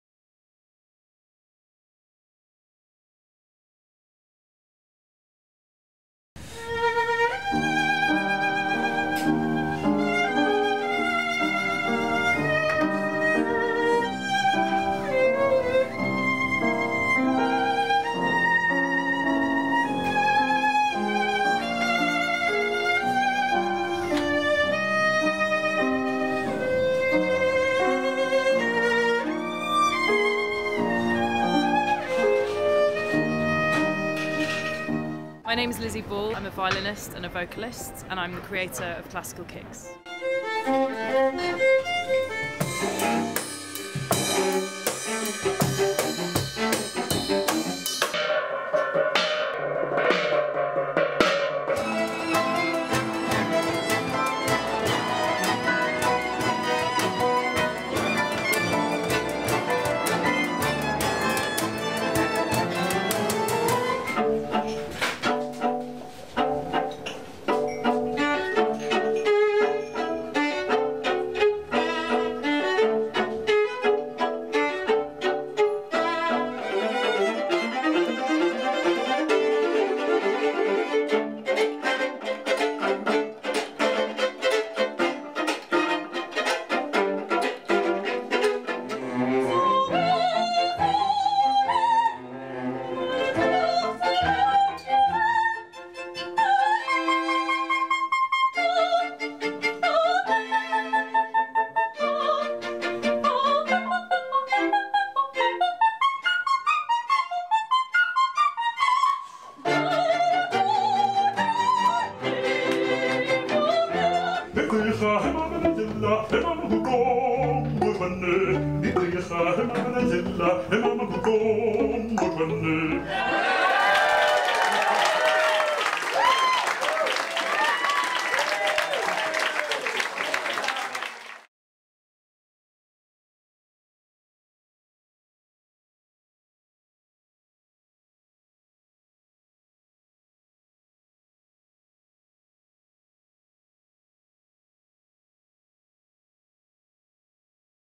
Classical Kicks EPK
Lizzie Ball, violinist and vocalist is proud to present the first ever Classical night at the world famous Ronnie Scotts Jazz Club, London UK. This is snapshot of the premiere on April 29th, 2012. Cinematographer: Heba Egamal, Sound Production: Sahan Satis